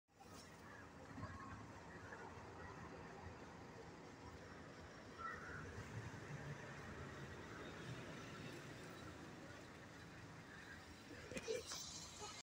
කවුද මේ එන්නෙ 👍👍🙏😃
HAY FRIEND,S
THIS IS AMAZING CTB BUS
COMES TO CTB BUSTAND
SRILANKA
COMES UNEXPECTEDLY